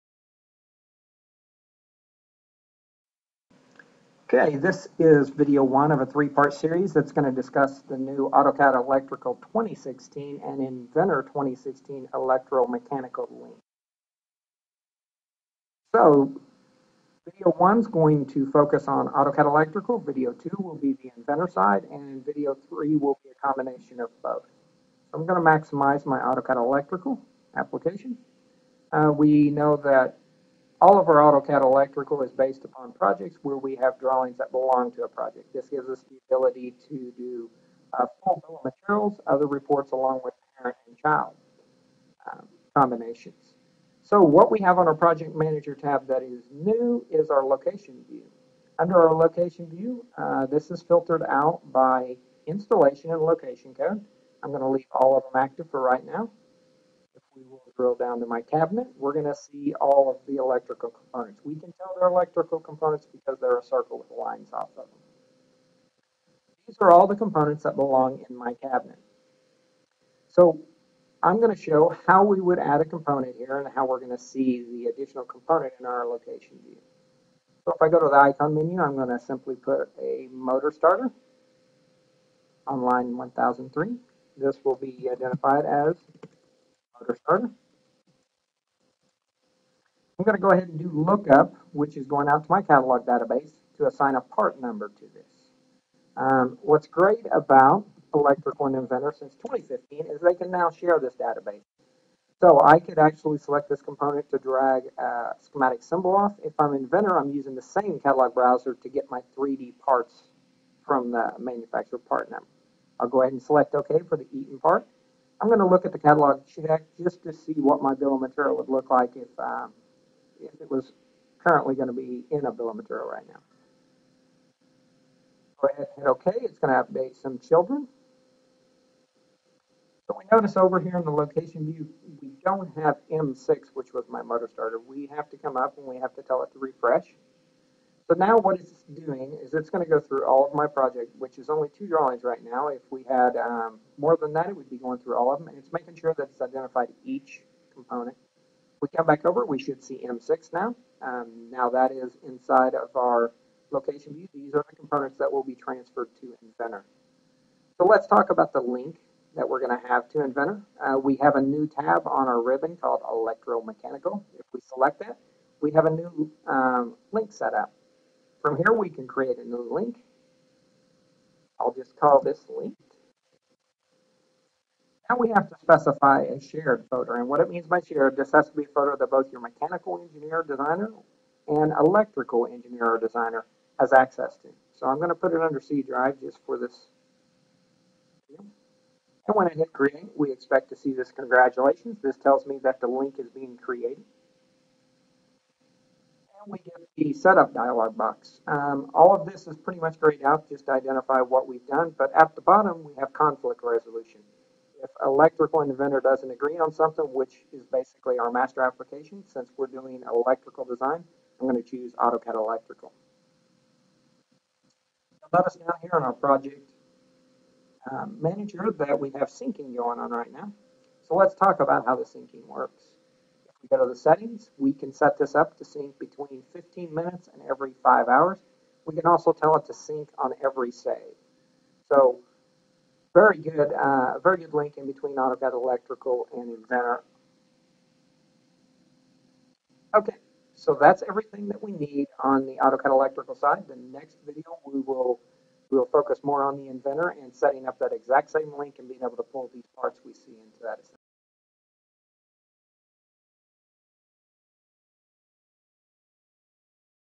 AutoCAD Electrical - AutoCAD Electrical 2016 Electromechanical (Part 1)
This video will show the new Electromechanical link in AutoCAD Electrical 2016